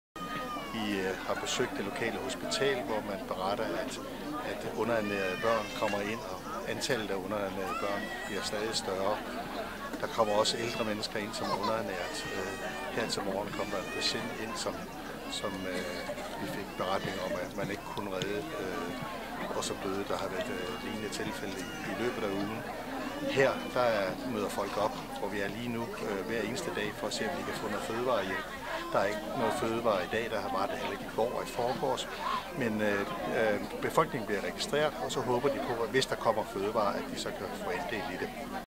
Tørken hærger Østafrika.
Vor mand i området, Søren Hougaard, er i de tørkeramte områder. Hør ham fortælle om manglen på mad.